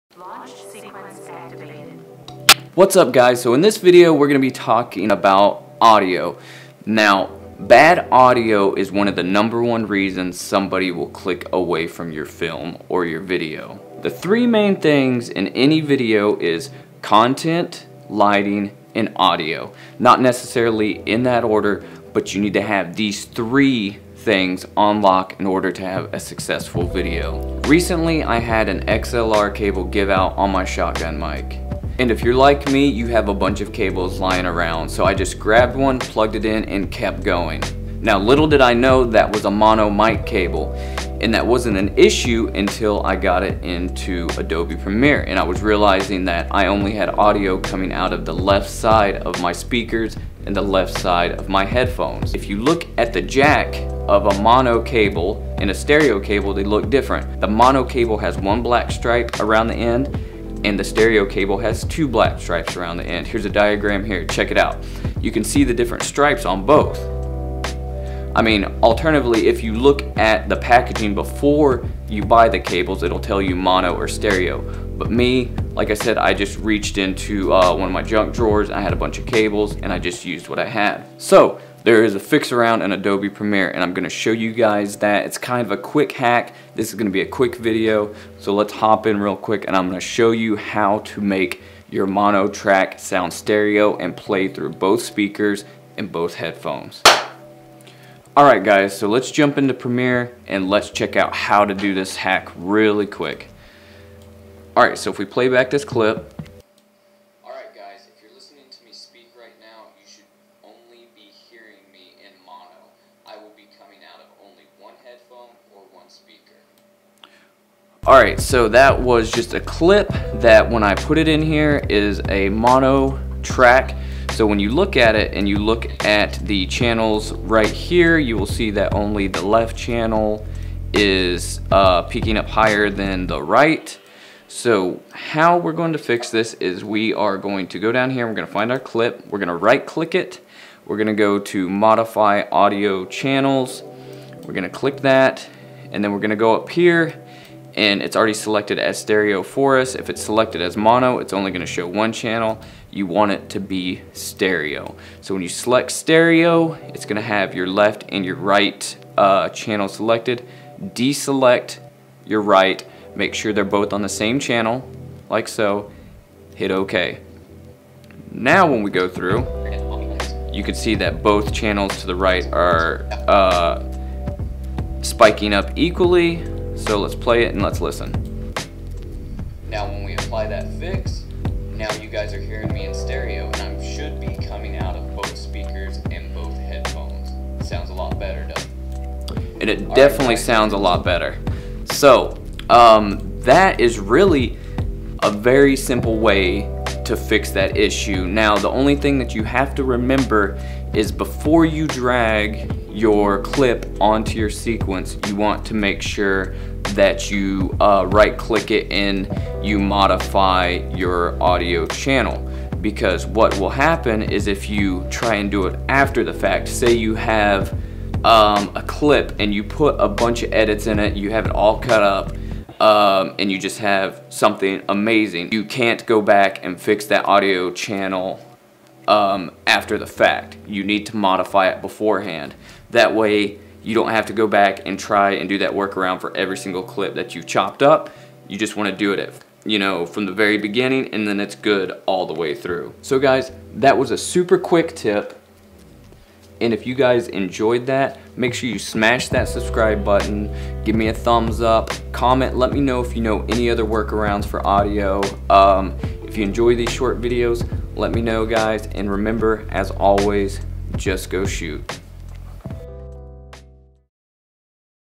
QUICK TIPS MONO TO STEREO IN PREMIERE 2018 & BEYOND
In this video, I am going to show you a QUICK TIP on how to CHANGE MONO TO STEREO IN ADOBE PREMIERE PRO CC 2018 & BEYOND.

Essentials:

Field Notes
Wide lens
On Camera Mic
Shotgun Mic
Samsung Note 8
Camera Bag